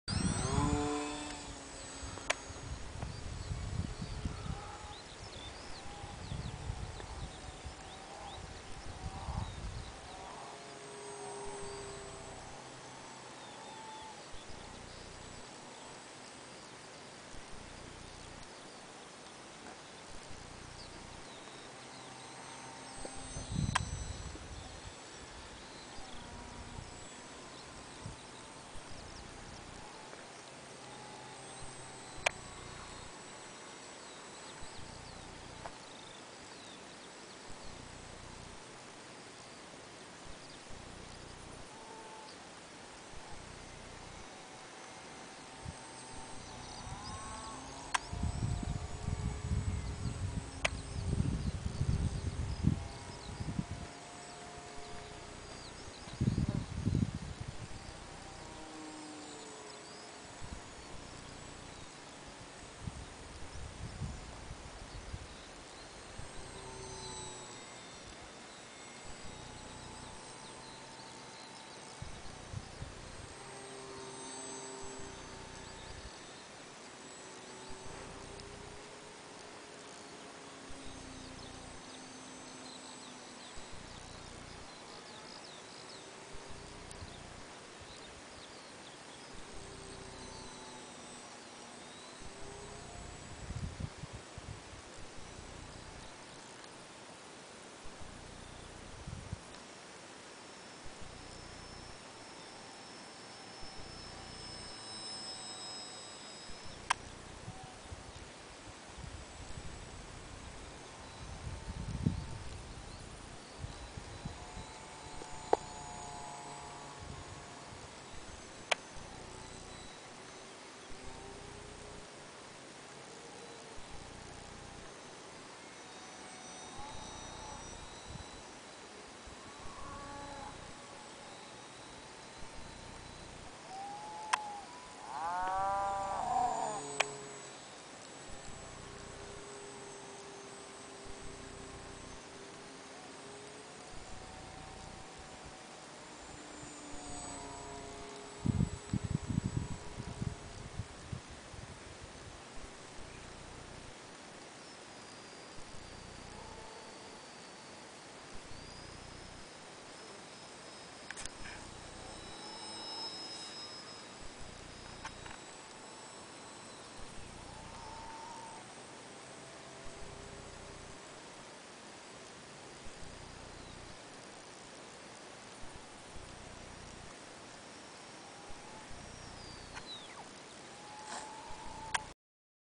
Hacker P-51 Mustang EPP second flight
Flight of HACKER P-51 Mustang made from EPP...Wingspann: 840mm, length: 660mm, RTF weight: 260g...Powered by HXT 24g 1500kv outrunner with GWS 8x4 HD prop, ESC TURNIGY PLUSH 10A, 2 x servo HXT 500 for ailerons and alevator, batterky TURNIGY nano-tech 3S 850mAh 25-50C..